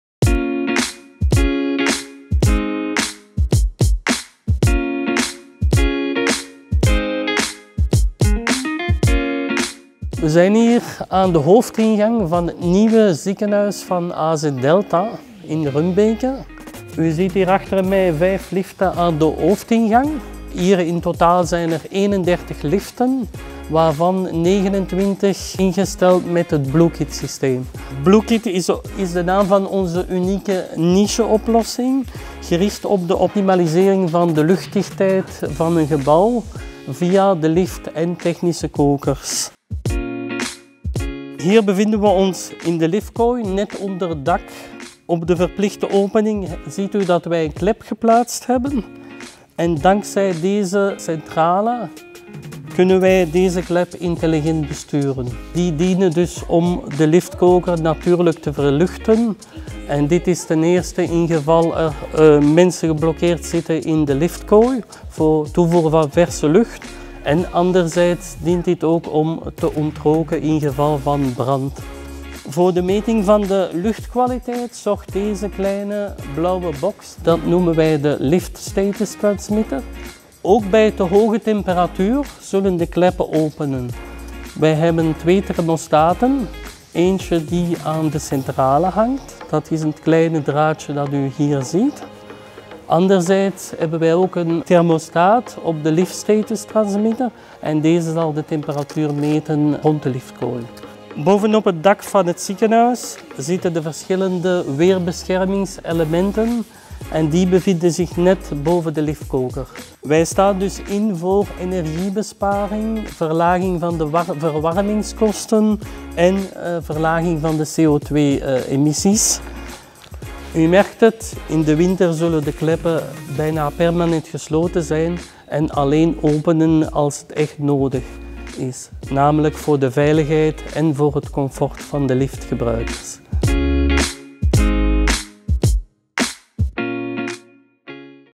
BlueKit levert diverse ontluchtingssystemen voor de liften van het nieuwe AZ Delta ziekenhuis
Voor het nieuwe AZ Delta ziekenhuis in Roeselare mocht BlueKit diverse ontluchtingssystemen leveren voor de liften. Dankzij dit systeem bespaart AZ Delta jaarlijks op haar stookkosten.